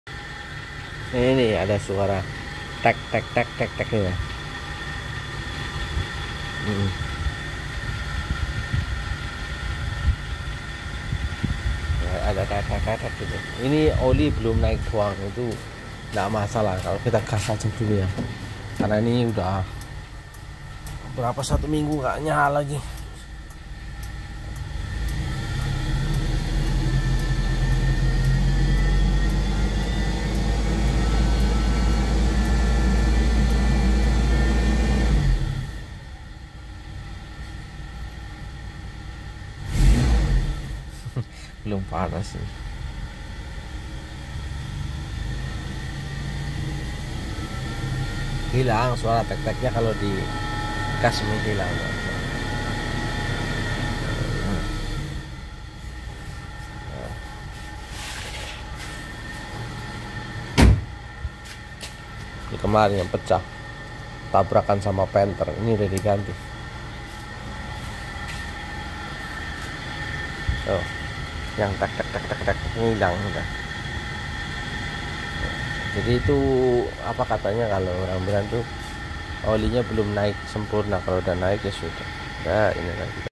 kijangku - Suara khas kijang Gasoline 7k LGX 1999 karburator platina bunyi tek tek saat dingin
kijangku -Sudah sejak 6 tahun lalu saat minang kijang ini suara tek tek tek dimesin kijang saat dingin awal dinyalakan , setelah oli melumasi bunyi kembali senyap. ngga masalah ya gaes pakai aja